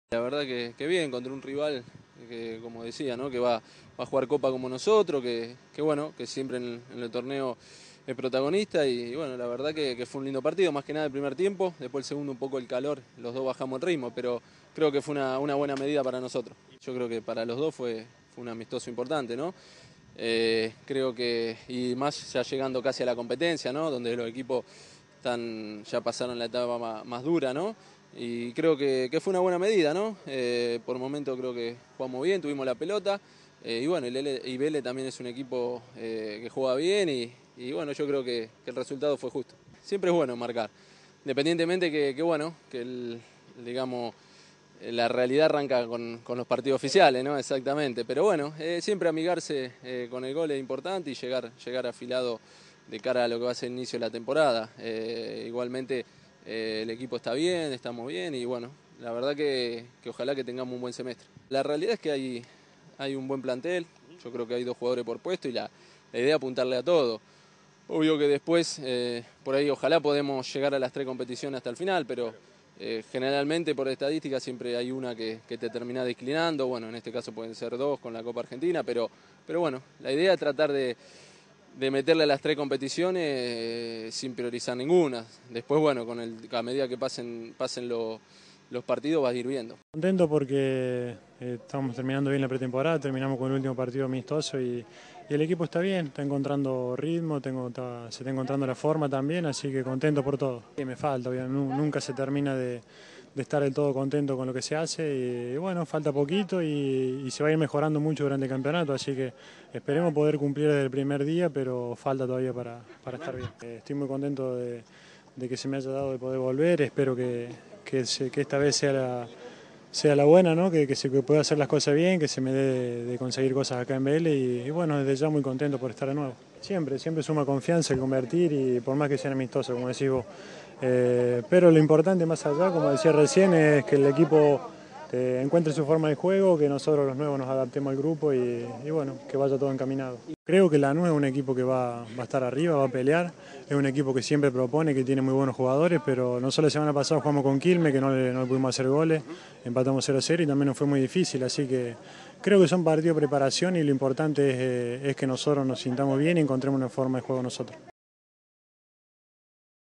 Velez y Lanús igualaron 2 a 2 en amistoso de pretemporada
Vélez Sarsfield y Lanús igualaron hoy en dos goles en un amistoso de pretemporada con miras al inicio de la actividad oficial de 2012 en la que ambos equipos disputarán el torneo Clausura, la Copa Libertadores y la Copa Argentina.

    Mauro Obolo, flamante incorporación de Vélez proveniente de Arsenal, en dos ocasiones y Mariano Pavone también por duplicado marcaron los goles en la igualdad.
Luego del encuentro, Obolo aseguró tener expectativas de "poder convertir desde el primer día" refiriéndose al comienzo de los torneos que le tocará afrontar a la institución de Liniers.
El ex Arsenal convirtió tres goles en los cuatro amistosos que disputó el 'fortín' desde el regreso a la Villa Olímpica de su pretemporada en Punta del Este donde hicieron la mayor parte del trabajo físico.
En dos ocasiones le marcó a Lanús en lo que fue el amistoso de preparación de mayor envergadura y el restante se lo anotó a Defensa y Justicia, "siempre suma confianza hacer goles por más que sean amistosos" dijo.
Por su parte, Mariano Pavone, sobre el partido, opinó que "para los dos equipos fue un amistoso importante, y creo que fue una medida". Sobre su actuación aseguro que "siempre amigarse con el gol es importante y llegar afilado de cara al inicio de la temporada".
    El encuentro se disputó en la Villa Olímpica de Vélez ubicada en la localidad bonaerense de Castelar y estuvo dividido en dos tiempos de 35 minutos, al tiempo que el árbitro fue Néstor Rodríguez Battaglia.
    Para Vélez, que contó en el segundo tiempo con la presencia de su otro refuerzo, Federico Insúa, significó su último partido amistoso antes del inicio oficial de la temporada.
    El conjunto dirigido por Ricardo Gareca debutará en el torneo Clausura el domingo 12 de febrero, a las 19, ante Godoy Cruz de Mendoza, en su cancha de Liniers, al tiempo que también tiene por delante en este primer semestre su participación en la Copa Libertadores y la Copa Argentina.
    En el torneo internacional hará su presentación el 7 de febrero como visitante ante Defensor Sporting de Uruguay por el grupo 7, mientras que por la Copa local jugará ante Racing de Trelew el 29 de este mes por los 32vos. de final.
    En tanto, Lanús tiene previsto un amistoso más antes del inicio de la actividad oficial ya que el sábado próximo se medirá ante un selectivo de Quilmes.
    El elenco "granate", conducido por Gabriel Schurrer, será el encargado de abrir el Clausura, ya que recibirá a San Lorenzo el viernes 10 a partir de las 21.15, mismo horario en el que jugarán el campeón Boca ante Olimpo de Bahía Blanca.
    Sin embargo, previamente, enfrentará el próximo martes 7 a Barracas Central por los 32vos. de la Copa Argentina, al tiempo que también disputará la Copa Libertadores, en la que jugará como local por el grupo 2 el 15 de febrero ante el ganador de la serie entre Flamengo de Brasil y Real Potosí de Bolivia, que se miden esta noche.